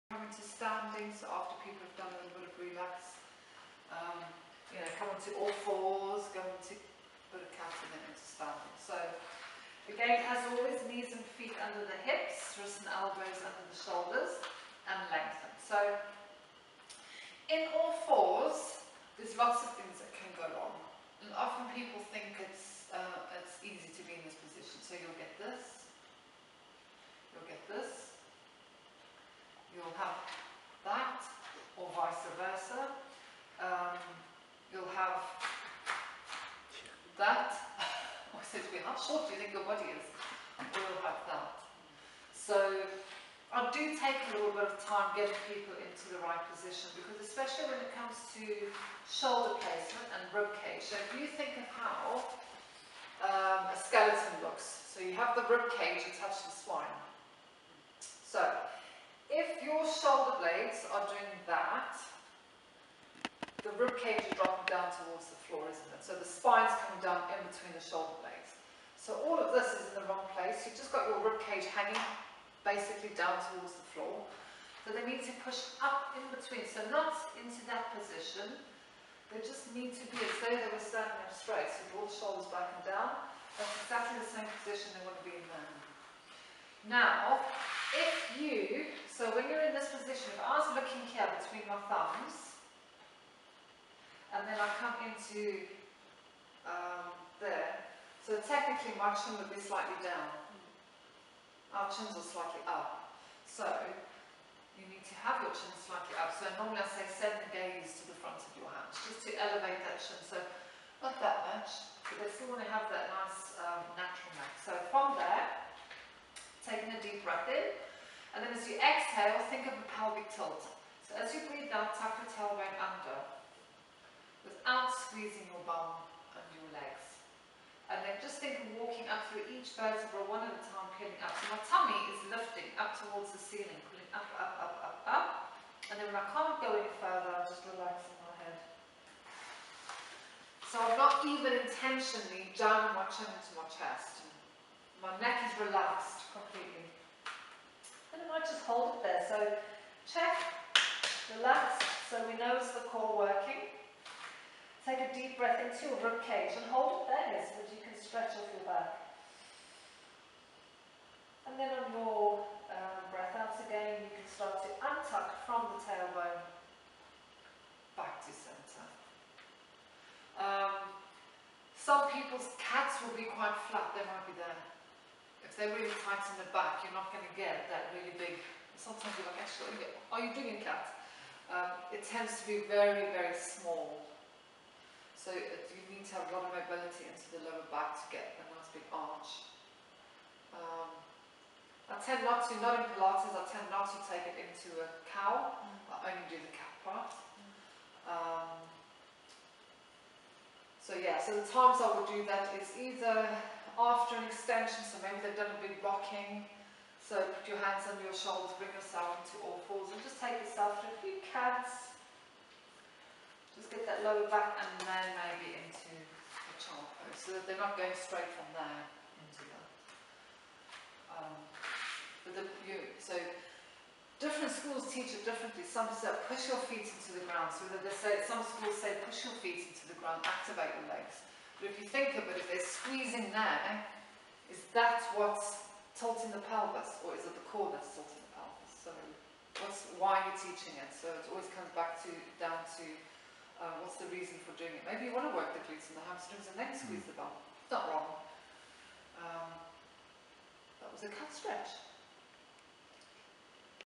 Pilates Cat
Pilates Teacher Training  -  Cat  (Work Book 1)
The Cat exercise is a spine mobilizing movement.
Instructor takes you through Cat stretch pilates movement Join my app for more classes, courses and Challenges